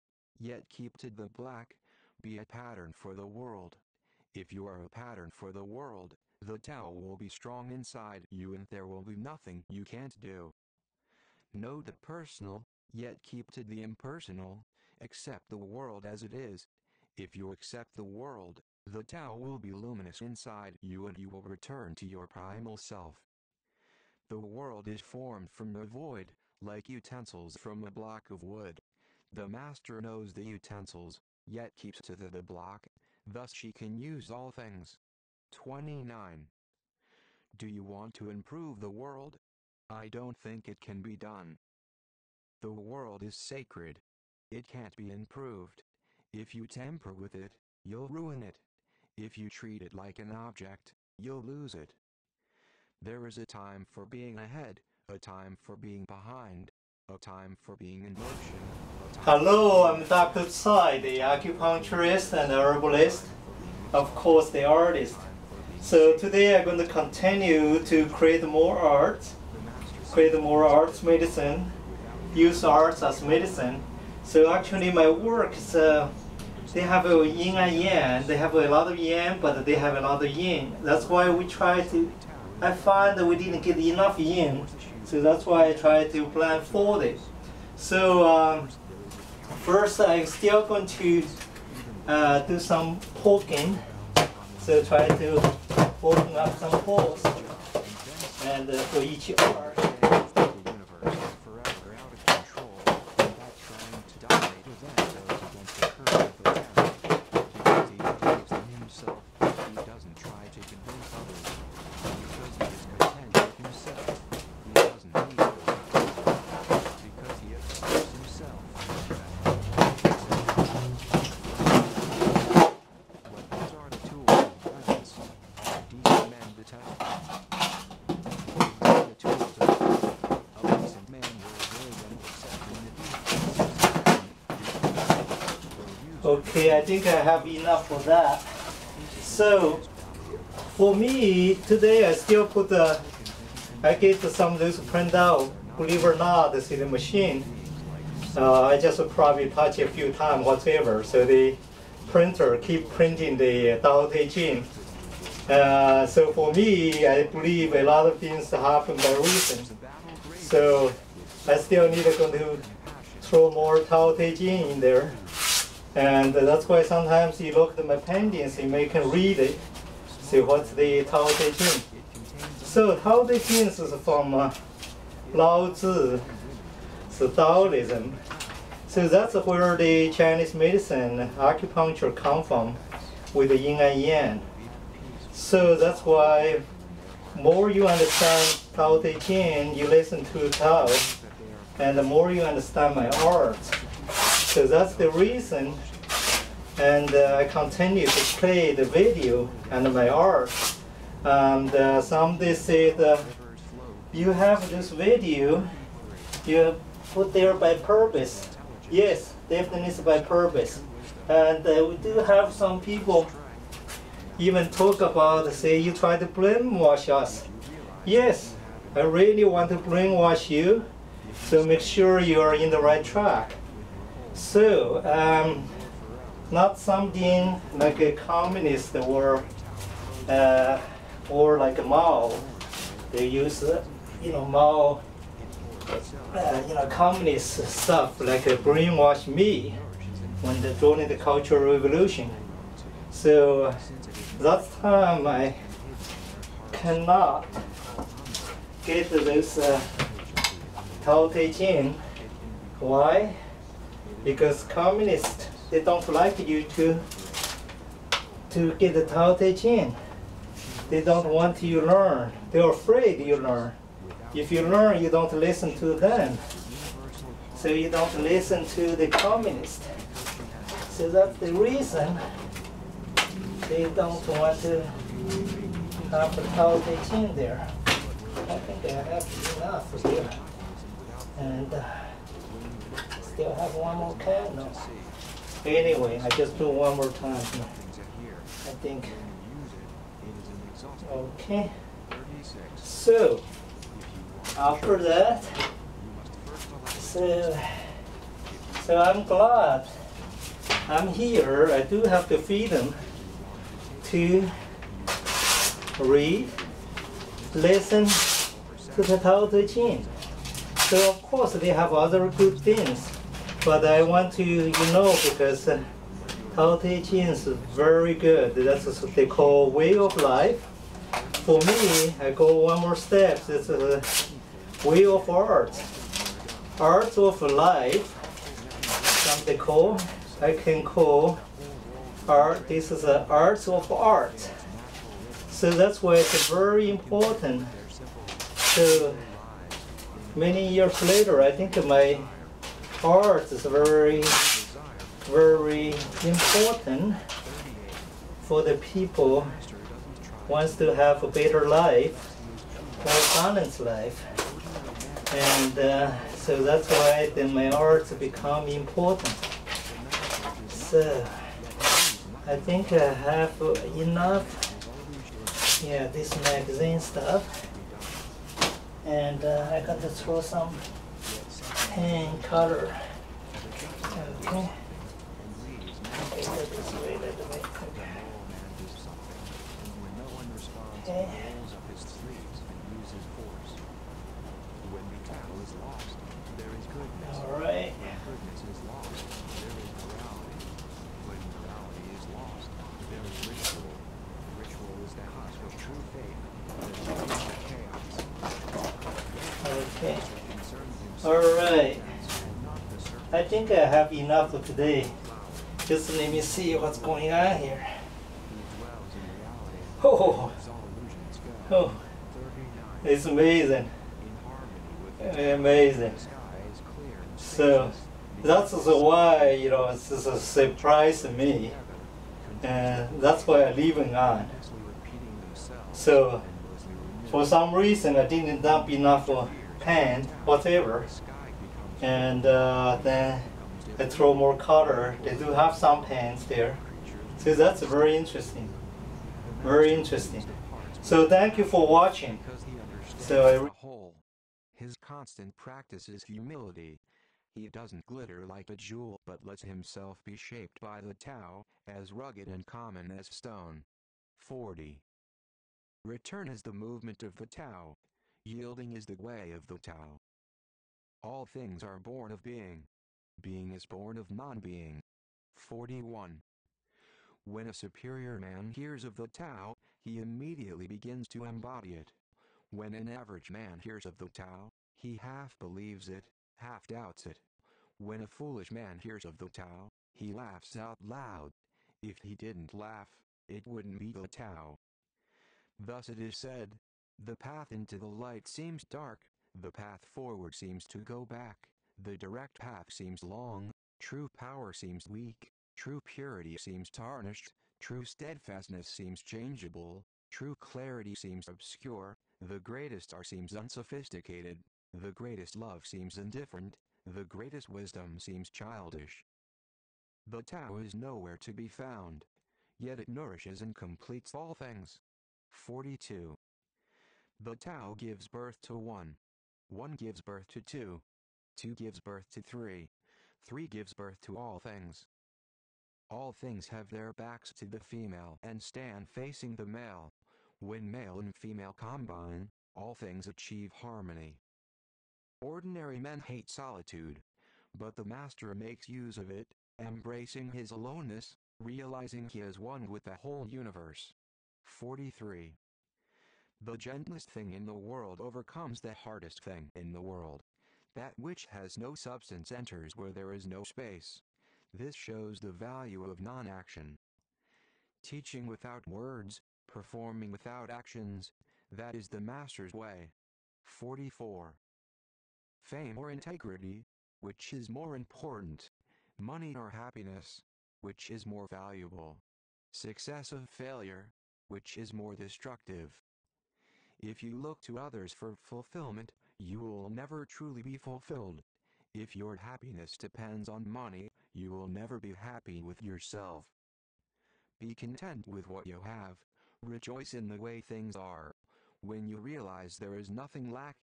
Jon Tsoi Master Of Tao-Blindfold Art Medicine Movement #293 - Fernando Luis Alvarez Gallery
Dr. Jon Tsoi Artist - Revolutionary-Cutting Edge-Contemporary blindfold art medicine movement. Creating art from inner vision, balance physically, mentally and spiritually.